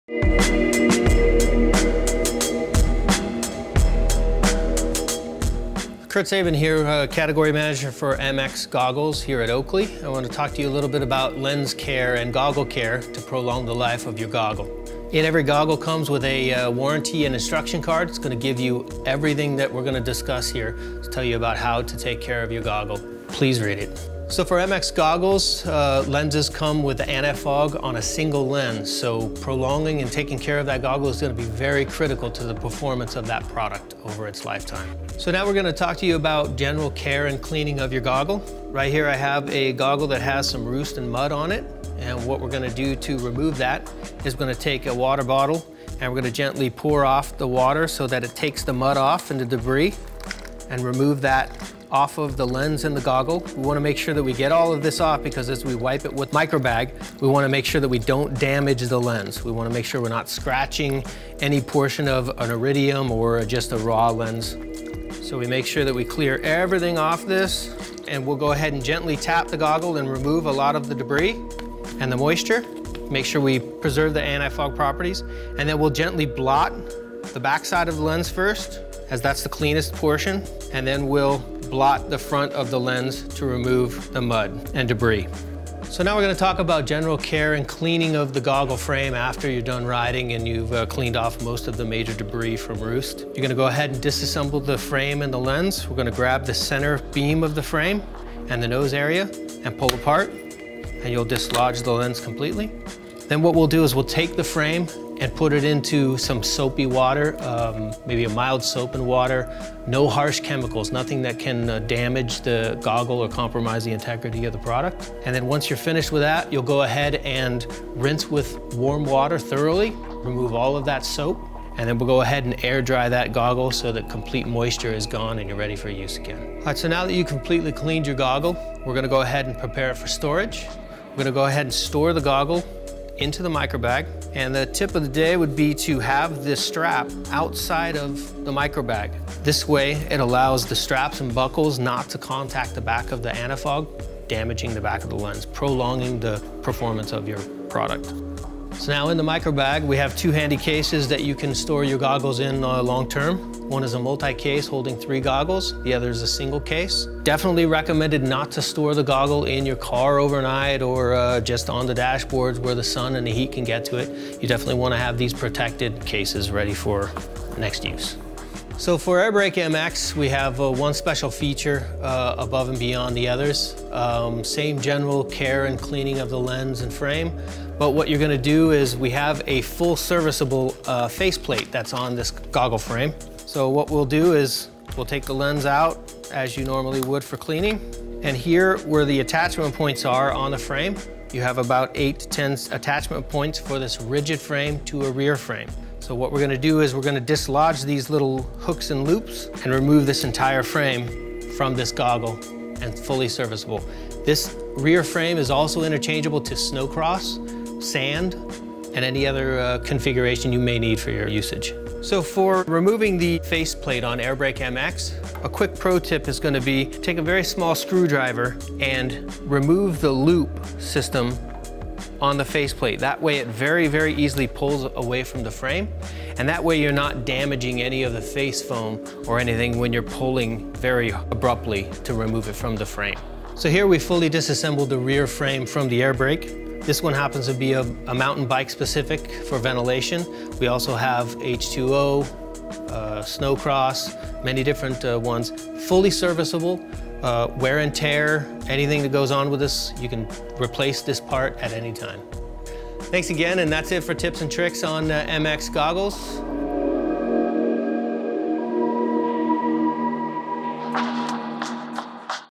MX GOGGLE CARE | OAKLEY PRO TIPS
Kurt Sabin, Goggle Product Category Manager, showcases how you can prolong the life of your Oakley MX Goggles & Lenses.  Know our products better with Oakley Pro Tips.